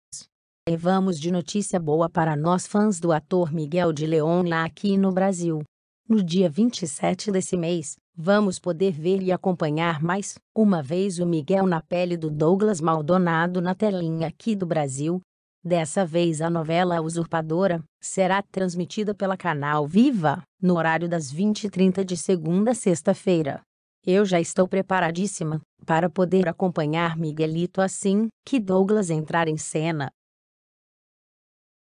Miguel De León de volta a telinha no Brasil
A partir do dia 27 vamos poder voltar acompanhar o Miguel na pele do Douglas Maldonado aqui no Brasil. Dessa vez a novela será transmitida pelo canal viva no horário das 20:30h. Só vem Douglas Maldonado!